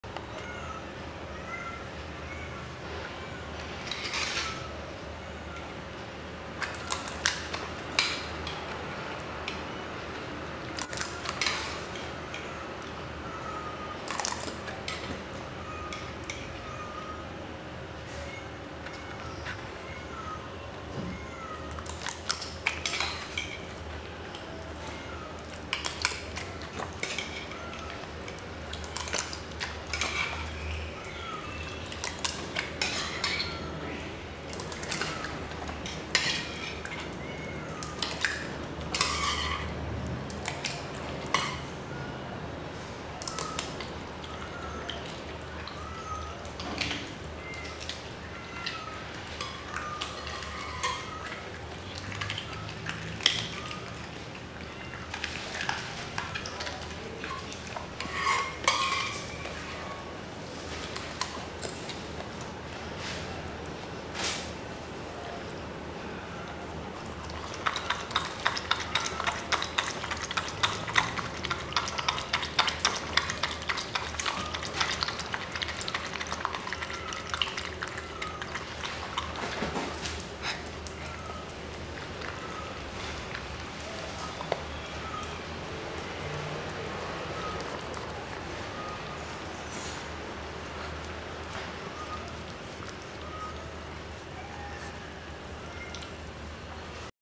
Velvet (miniature schnauzer) eating Organic RAW Meats/Bones/Offals/Healthy Powder/Green Mush
Velvet - 5kg hand-stripped miniature schnauzer

Organic RAW foods for dogs & cats